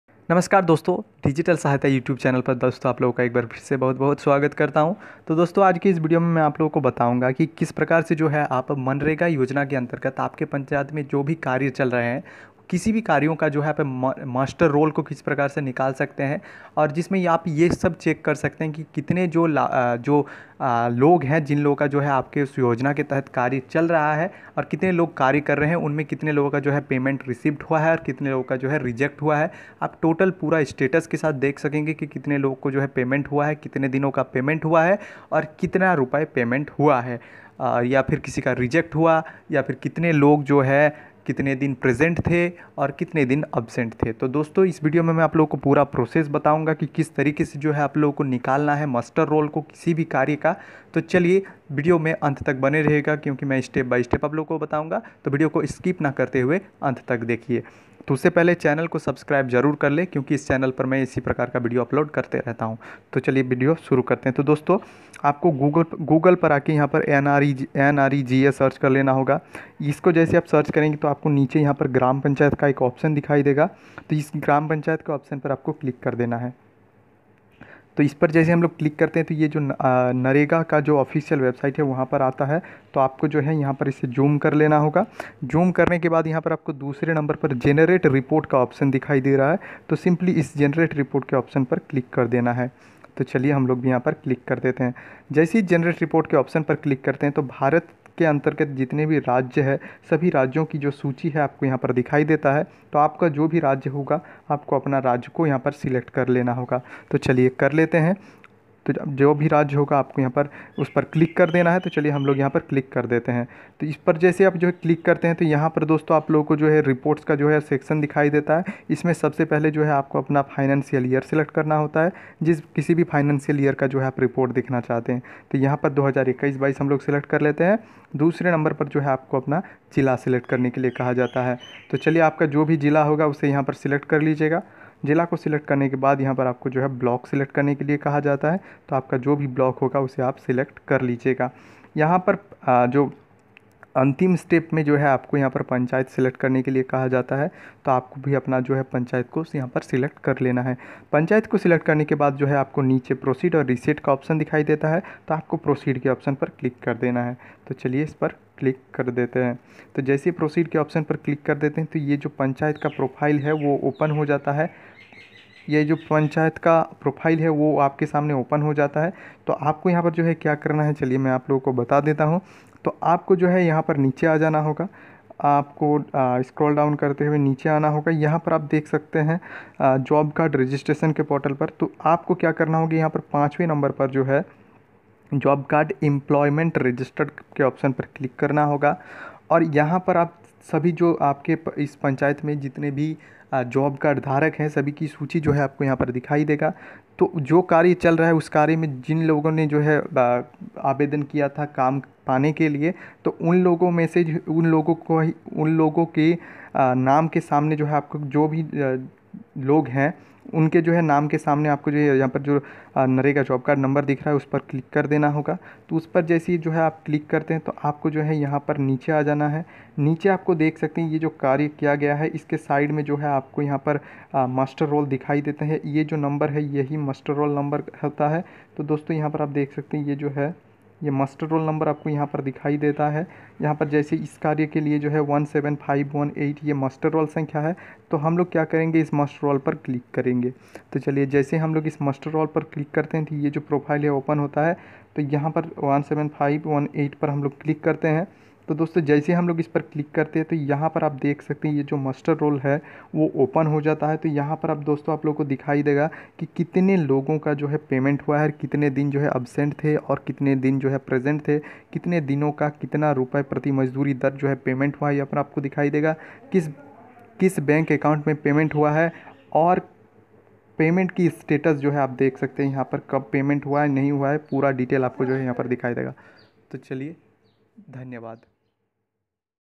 मनरेगा मास्टर रोल कैसे निकाले | Mustur Roll Kaise dekhe | nrega muster roll 2021
Maine is video mai muster roll kaise dekhe Or nrega muster roll kaise nikale bataya hai

Quries Solved

• Mustur Roll Kaise dekhe
• muster roll kaise nikale
• nrega muster roll kaise nikale
• muster roll
• nrega
• mgnrega muster roll
• nrega com